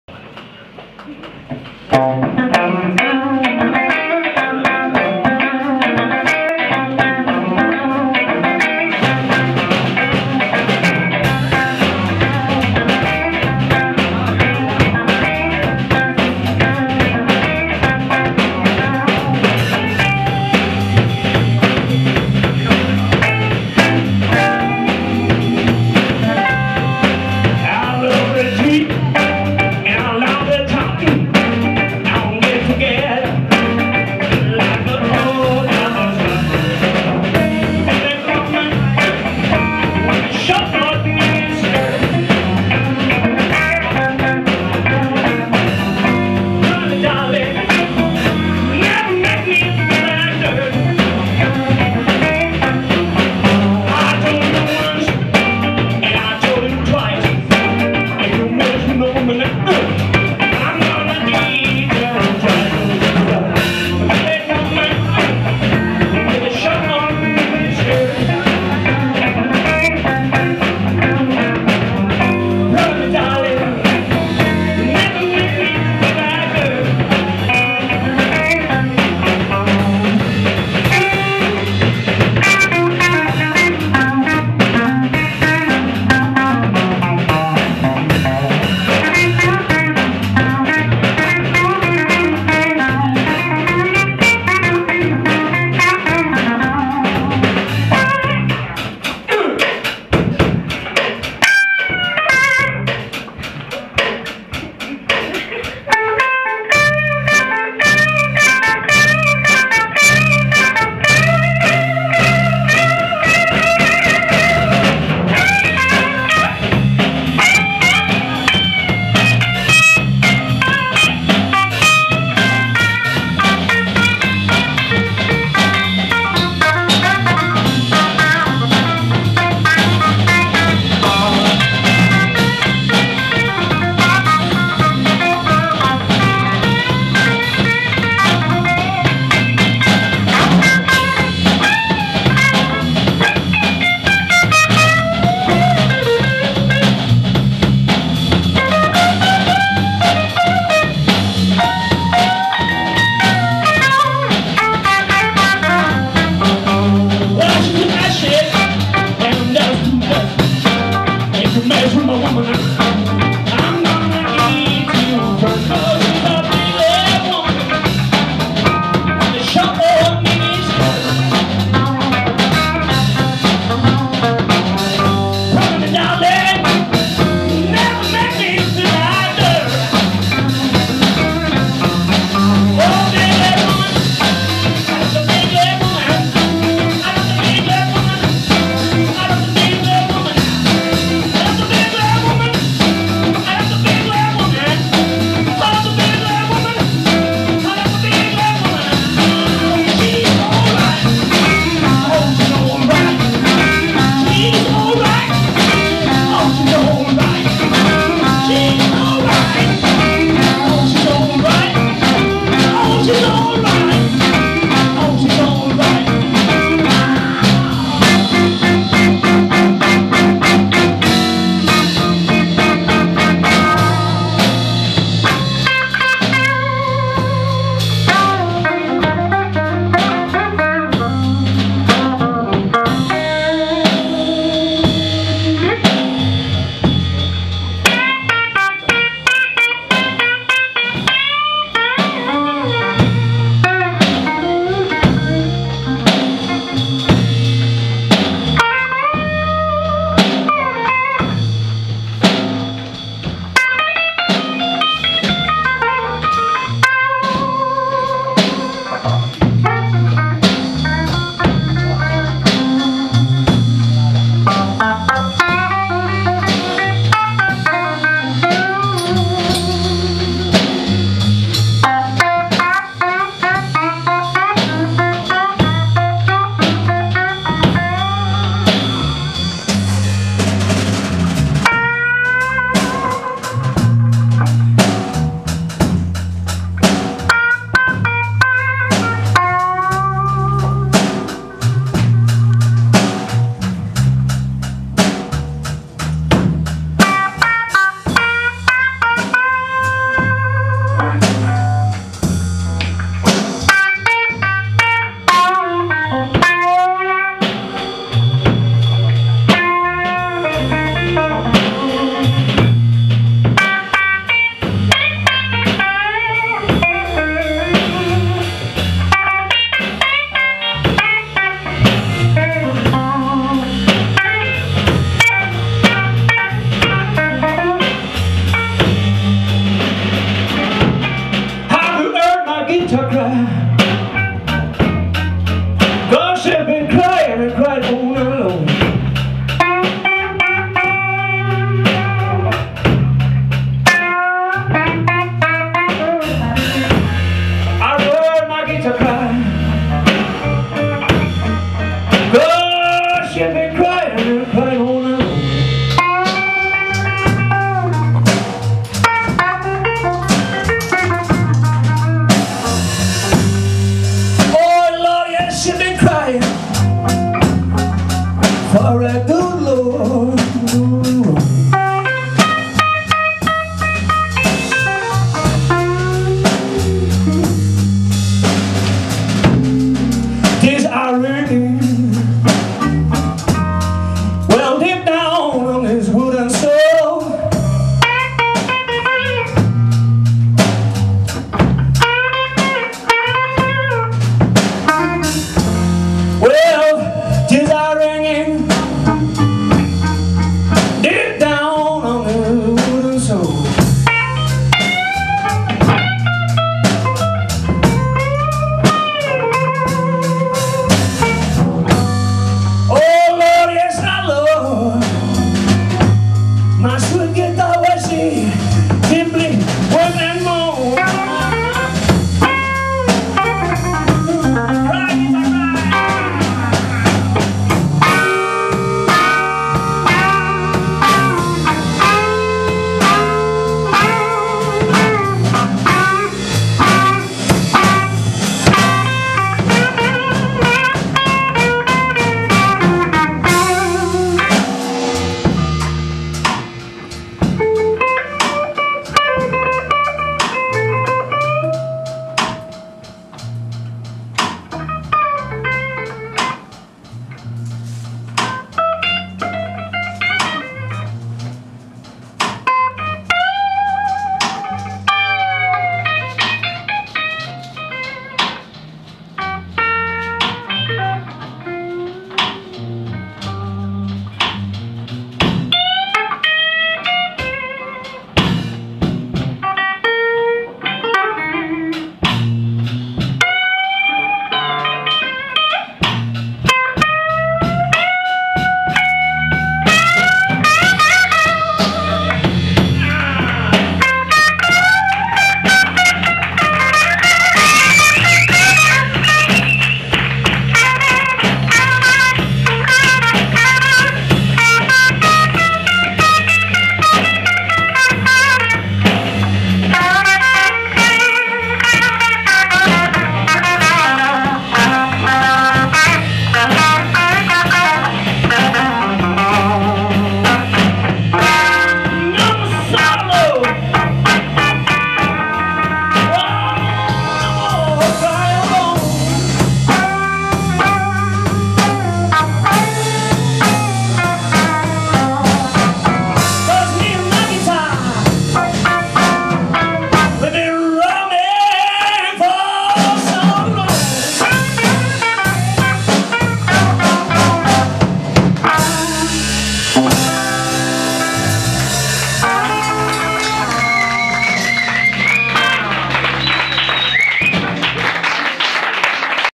Station Road
Station Road en el concierto jam del domingo 17 de junio organizado por la SBM.